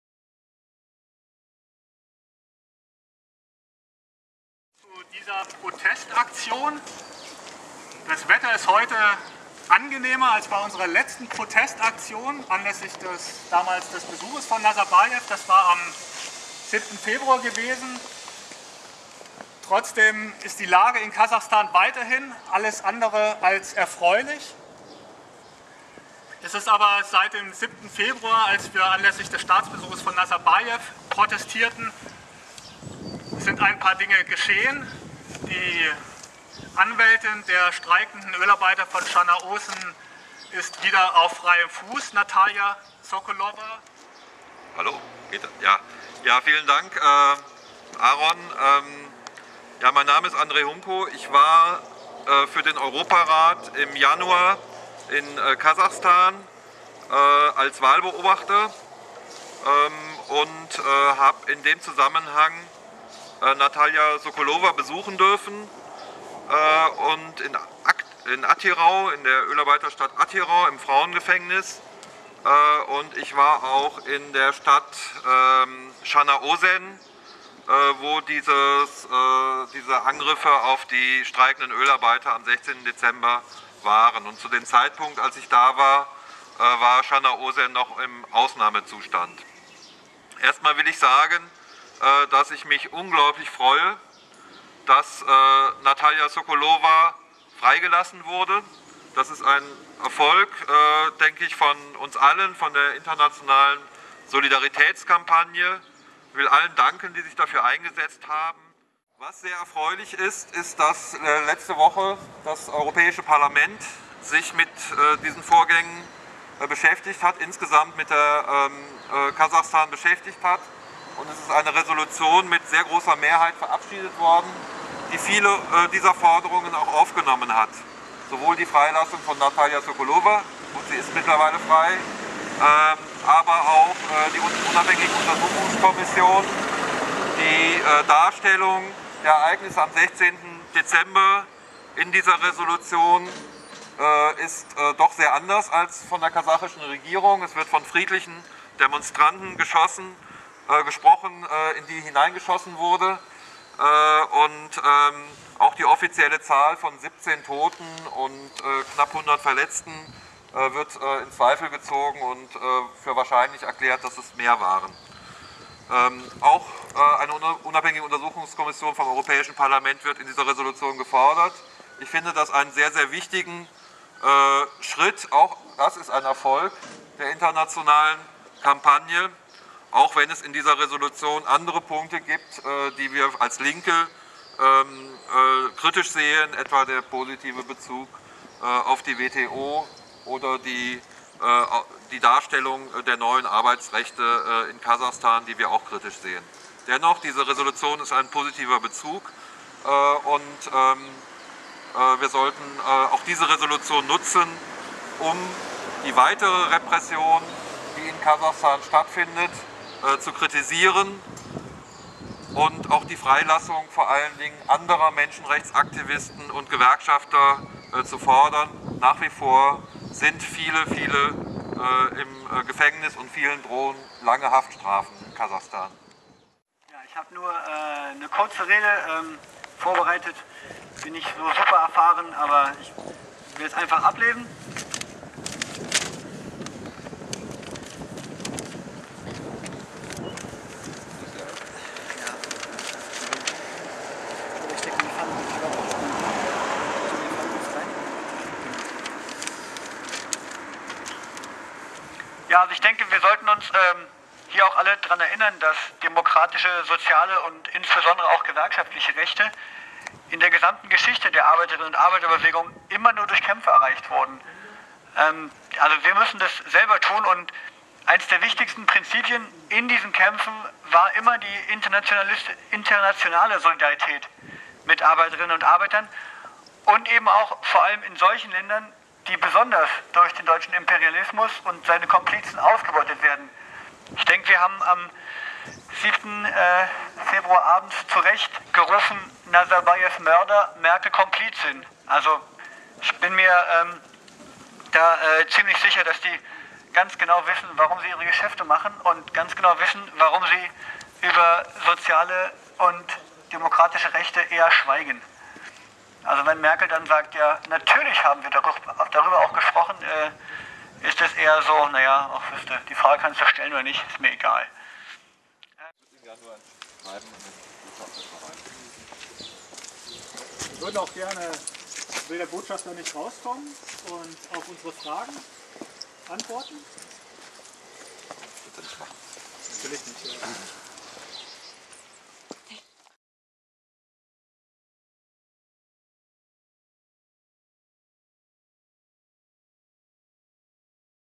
Soliaktion vor kasachischer Botschaft 21. März 2012 in Berlin
Soliaktion in Berlin im Rahmen der internationalen Aktionswoche von campaignkazakhstan